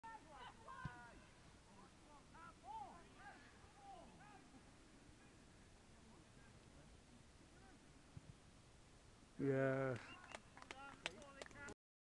PRE-SEASON FRIENDLY ST COLUMB 0 - 4 ST STEPHEN
St Stephen go 3-0 up in the 2nd half.